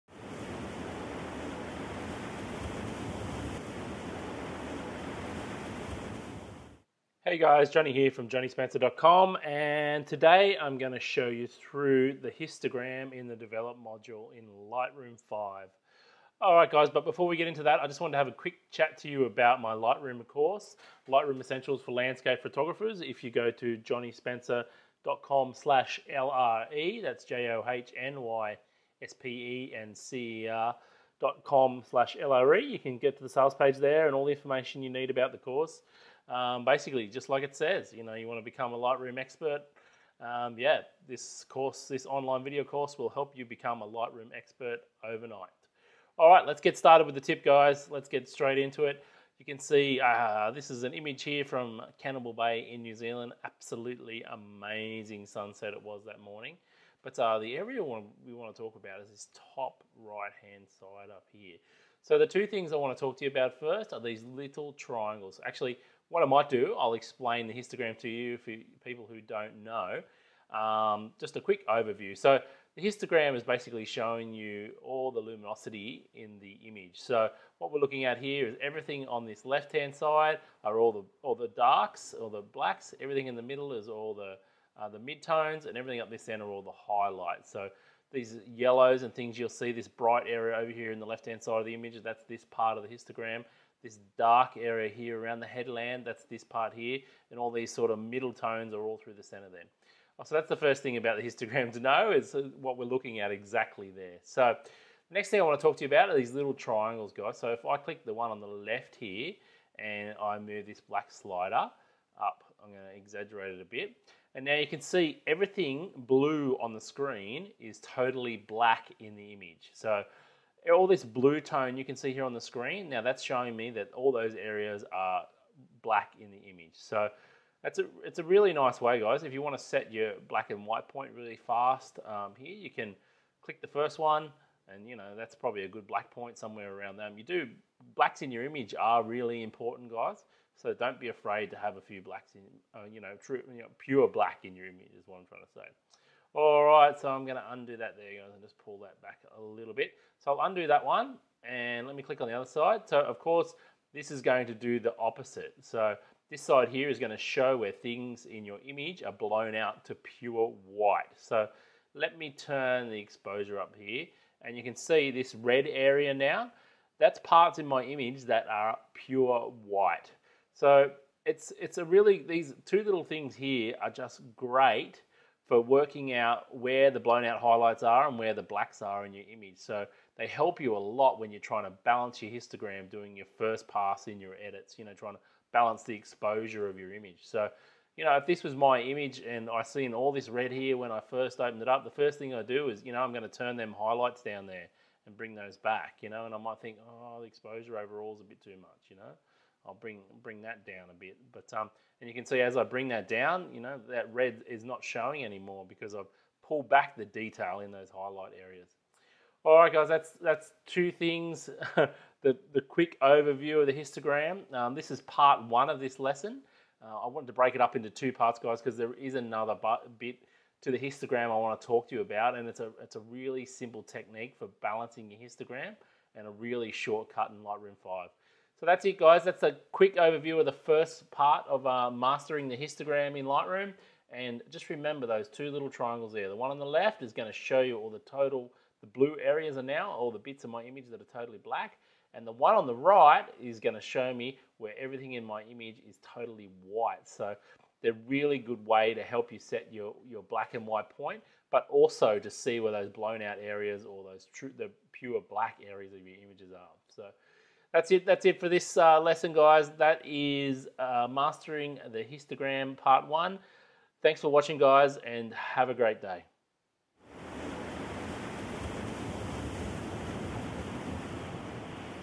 Lightroom - Mastering the Histogram Part 1
In this video you'll learn about the histogram in the Adobe Lightroom.